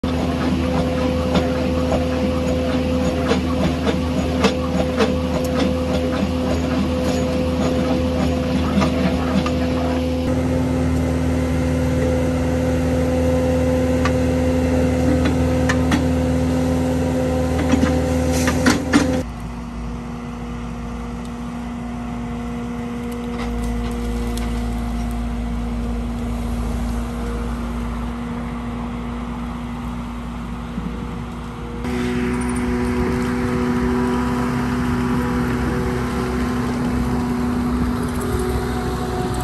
Drilling more rock & moving brush with BOBCAT T650 Skid Steer bobcatequipment 15c auger
Using the bobcat, T650 skid steer with a 15 C ￼bobcat auger 12 inch bit with Pingo rock head to drill through rock for a gate post for an entrance gate. We will also be using the bobcat 72 inch root grapple to clean up some trees we had to cut down that were in the way of the fence. Bobcatequipment Working Construction Auguring RockDrill 1ToughAnimal Property maintenance